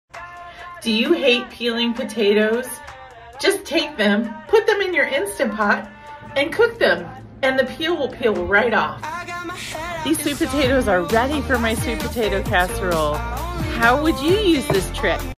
Do you hate peeling potatoes? My favorite hack for easy mashed potatoes or sweet potatoes.
Do you hate peeling potatoes?  As someone with Celiac Disease, potatoes and sweet potatoes are a staple in my diet.  I hate peeling potatoes.

Cutting them is challenging sometimes too. My favorite hack is to place them in the InstantPot with a half cup of water and the steaming rack.  I cook on manual for 10 minutes or more depending on the number and size of the potatoes.

Let the pressure release naturally.  Check to make sure they are completely cooked.

To pull the peel off my hand, let the potatoes cool before handling.  Remove carefully with a spoon if they are still hot.

Follow for more tips on living gluten free with Celiac Disease. @sukipwd